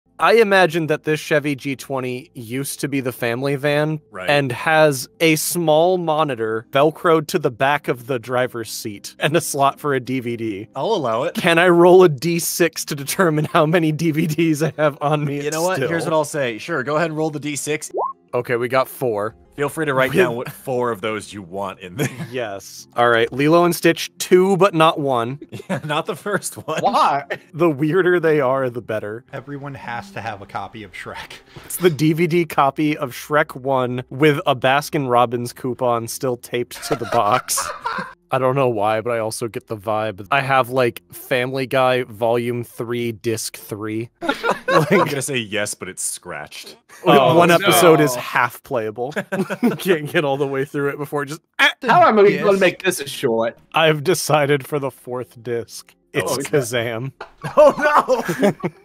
Digi-Destiny: Gary's DVD Collection
Gary is always prepared for a road trip. What other DVDs do you think he has?